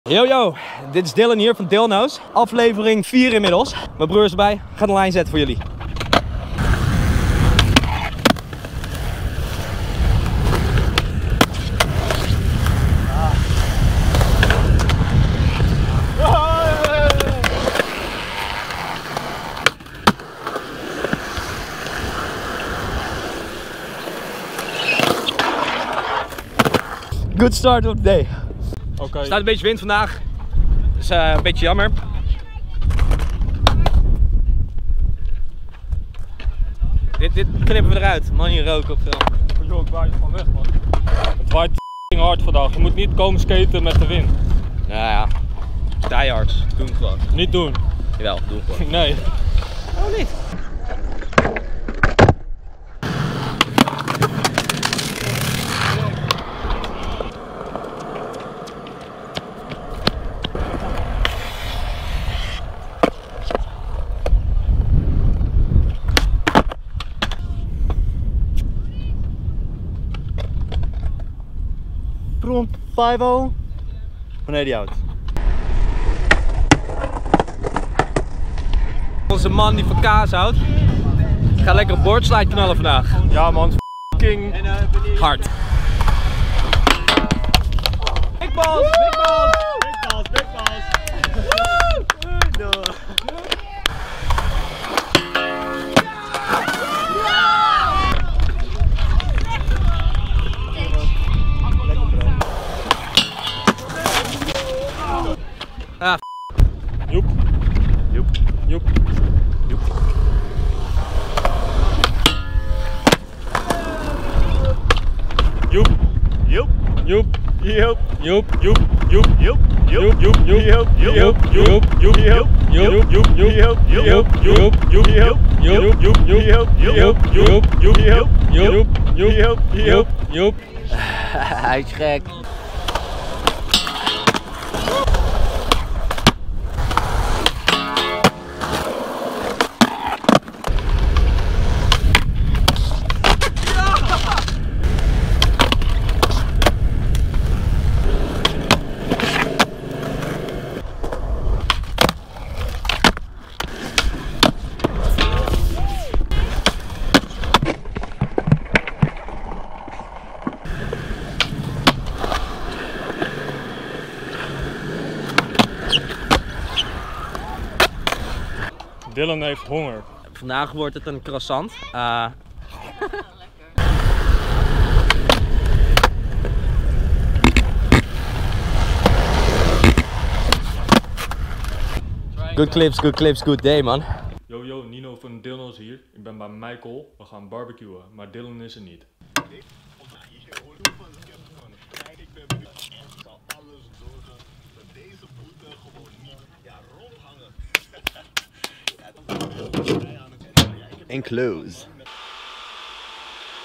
Joep | Hoofddorp Floriande | VLOG #4
Deze keer zijn we weer aangekomen bij skatebaan Hoofddorp Floriande, we wouden een park overview doen maar uiteindelijk is er wat anders uitgekomen..

Naja, het mannetje wat de beschrijvingen invult heeft vandaag even geen zin, de video spreekt voor zichzelf, bij de volgende video weer een wat uitgebreidere beschrijving.